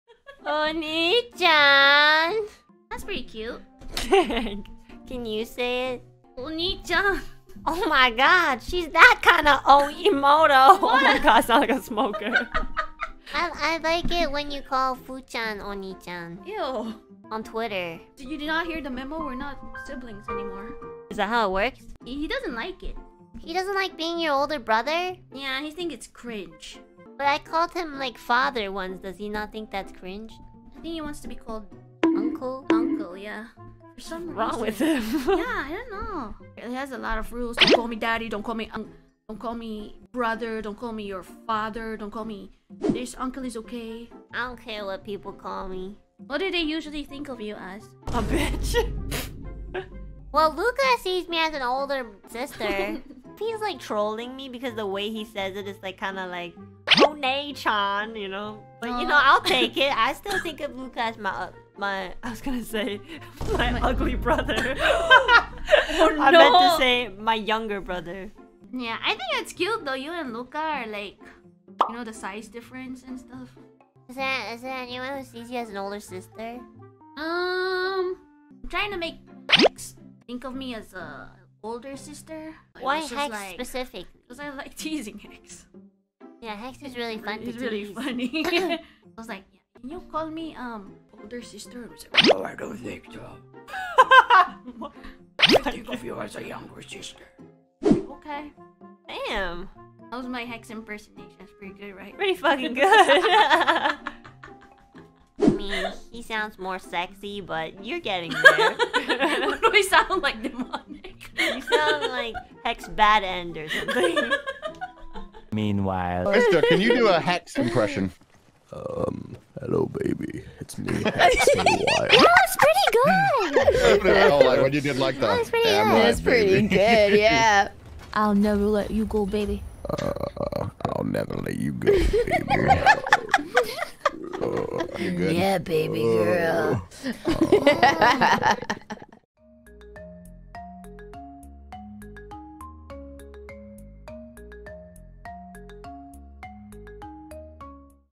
Millie & Enna have sisterly issue..
I share some Nijisanji EN clip that I find funny
mostly the trio bestie "Elira Pendora, Enna Alouette and Millie Parfait"